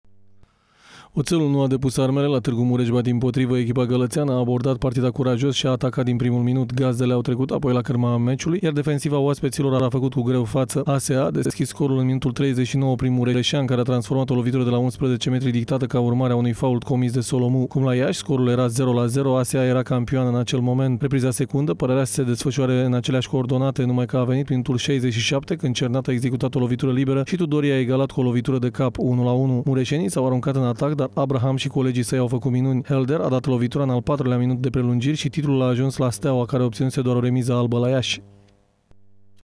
SPORT / Știre Etv - Oțelul a stabilit campioana | 29.05.2015
Oțelul a reușit să producă prima înfrângere a celor de la ASA Tg. Mureș pe propriul teren. Echipa gălățeană a revenit de la 0-1 și a câștigat cu 2-1 prin golurile marcate de Tudorie și Helder. În urma rezultatului înregistrat la Tg. Mureș, Steaua a cucerit al 3-lea titlu consecutiv.